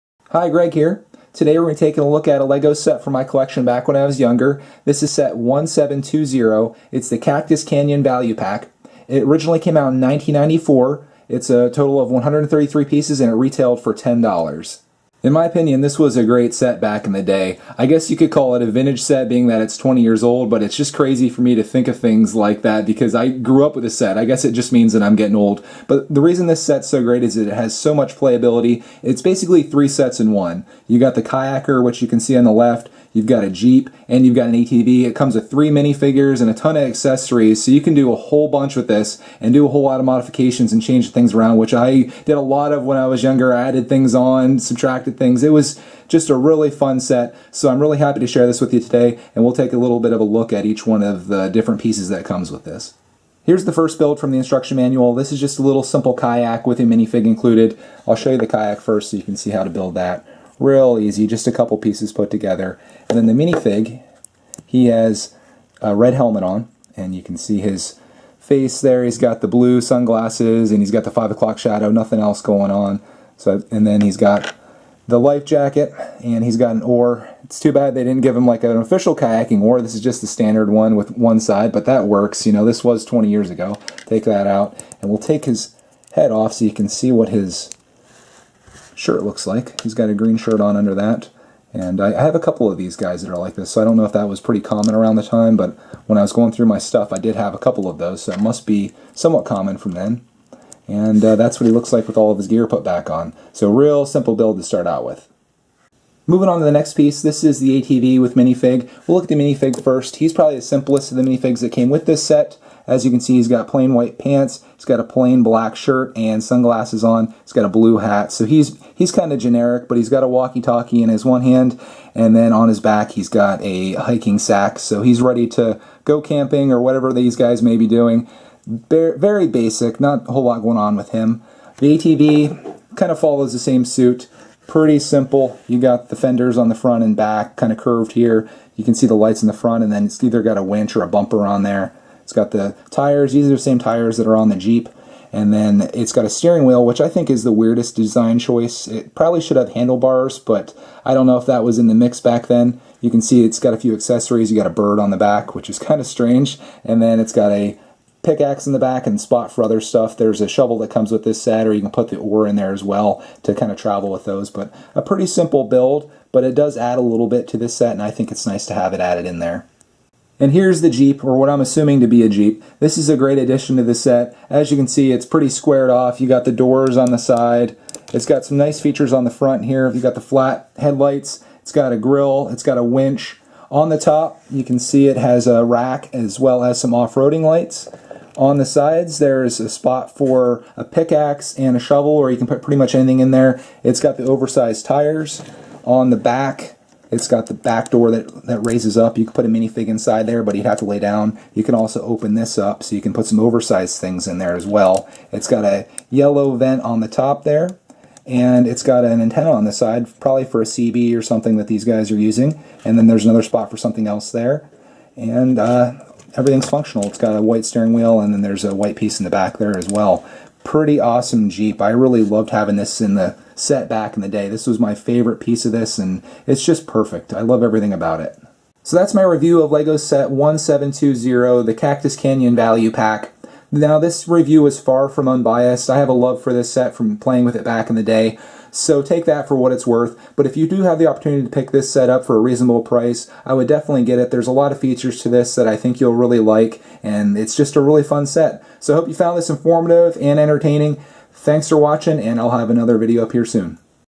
Vintage LEGO 1720 Cactus Canyon Value Pack Review
I review a vintage LEGO set from 1994.  This is set 1720 Cactus Canyon Value Pack.  It features a Jeep/SUV, an ATV, and a kayak with three minifigures.